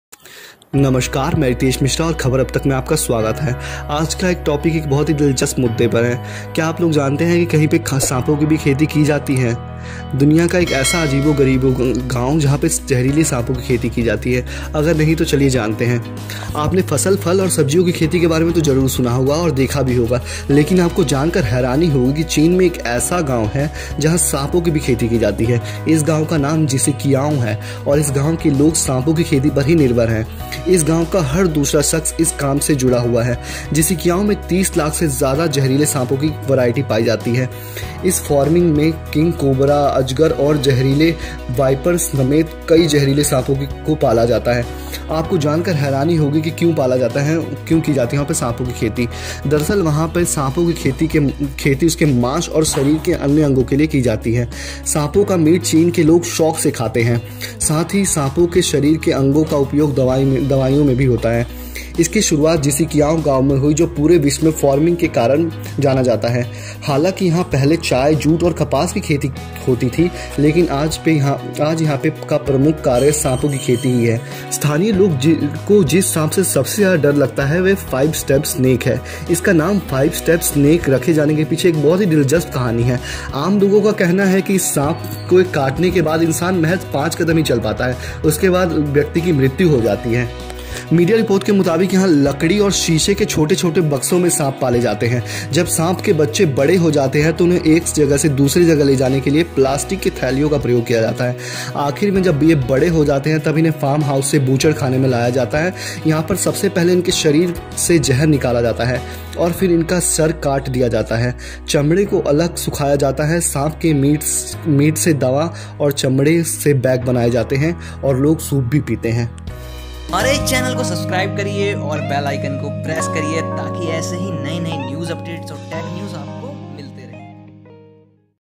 China secrets food manufacturing||China snake farm||The strange village if China having snakes farm.
China secrate food manufacturing
China snake from
The strange village if China having snakes farm
The strange village of China having snakes farmingchina snake farmer,

दुनिया का वो अजीबोगरीब गांव, जहां होती है जहरीले सांपों की खेती

फसल, फल और सब्जियों की खेती के बारे में आपने जरूर सुना होगा। लेकिन आपको यह जानकर हैरानी होगी कि चीन के एक गांव में सांपों की खेती की जाती है। जिसिकियाओ नाम के इस गांव के लोग सांपों की खेती पर निभर्र हैं। इस गांव का हर दूसरा शख्स इस काम से जुड़ा हुआ है। जिसिकियाओ में 30 लाख से ज्यादा जहरीले सांपों की वैरायटी पाई जाती है। इस फार्मिंग में किंग कोबरा, अजगर और जहरीले वाइपर समेत कई जहरीले सांप पाले जातेa

ये है सांप की खेती की वजह
यहां सांप की खेती उनके मांस और शरीर के अन्य अंगों के लिए की जाती है। सांप का मीट चीन में शौक से खाया जाता है। साथ ही सांपों के शरीर के अंगों का उपयोग दवाओं के निर्माण में भी

यूं हुई शुरुआत
जिसिकियाओ गांव पूरे विश्व में स्नेक फार्मिंग के कारण जाना जाता है। हालांकि यहां पहले चाय, जूट और कपास की खेती होती थी, लेकिन आज यहां प्रमुख कार्य सांप की खेती है।
फाइव स्टेप स्नेक से डरते हैं यहां के लोग
स्थानीय लोगों को जिस सांप से सबसे ज़्यादा डर लगता है वो है फाइव स्टेप स्नेक। इसका नाम फाइव स्टेप रखे जाने के पीछे भी बड़ी दिलचस्प कहानी है। आम लोगों का मानना है कि इस सांप के काटने के बाद इंसान महज पांच कदम चल पाता है और उसकी मौत हो
मीडिया रिपोर्ट के मुताबिक यहां लकड़ी और शीशे के छोटे-छोटे बक्सों में सांप पाले जाते हैं। जब सांप के बच्चे अंडों से निकल कर बड़े हो जाते हैं तो उन्हें एक जगह से दूसरी जगह ले जाने के लिए प्लास्टिक के थैलों का प्रयोग किया जाता है।
आखिर में सांप जब बड़े-बड़े हो जाते हैं तो फार्म हाउस से उन्हें बूचड़ खाने ले जाया जाता है, जहां सबसे पहले इनके जहर को निकाला जाता है और फिर इनका सर काट दिया जाता है। इसके बाद सांपों को काटकर उसका मीट निकालकर अलग रख लिया जाता है। चमड़े को अलग सुखाया जाता है। सांप के मीट से दवा और चमड़ों से बैग बनाए जाते हैं।